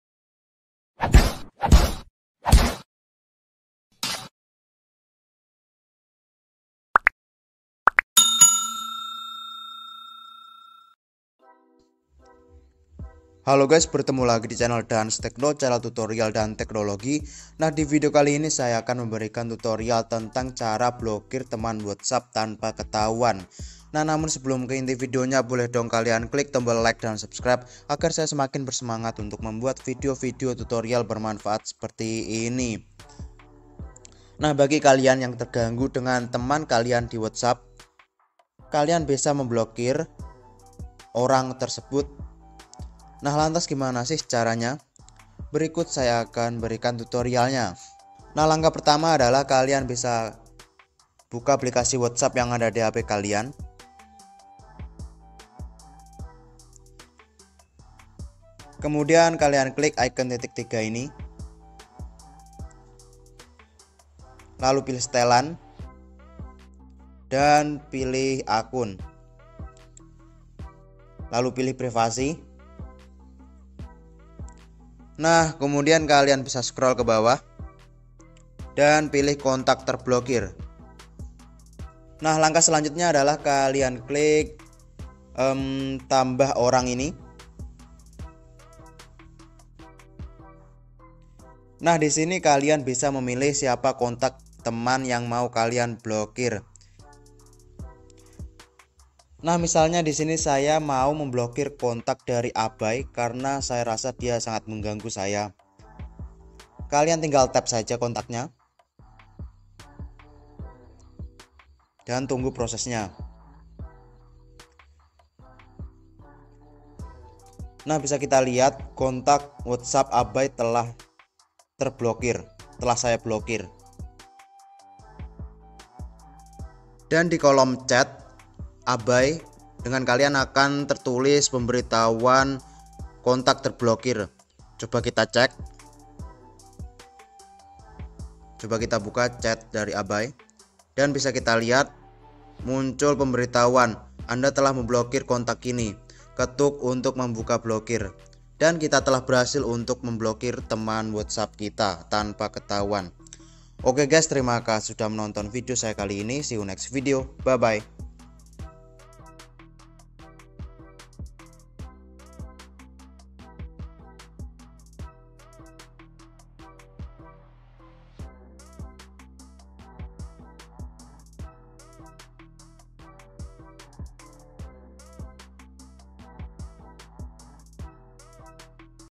Cara Blokir Kontak WhatsApp Tanpa Diketahui || Tutorial WhatsApp Terbaru
✨Hallo guys✨
Jadi divideo ini saya akan memberikan tutorial tentang Cara Blokir Kontak WhatsApp Tanpa Diketahui
😎 .Nah untuk kalian yg penasaran gimana caranya makanya tonton video ini sampai habis👌

Happy Watching🤗

•Record: xrecorder
•Thumbnail: Pixellab
•Editing: openshot